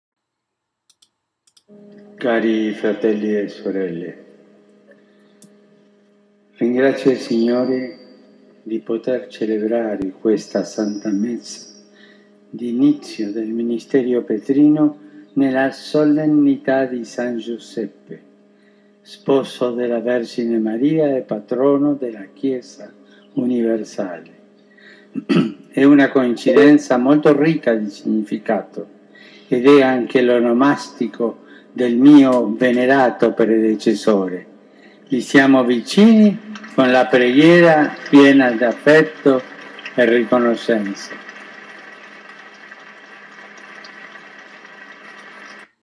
Papa Francesco ringrazia Papa Benedetto XVI
Papa Francesco ringrazia Papa Benedetto XVI durante l'omelia della messa di inizio pontificato.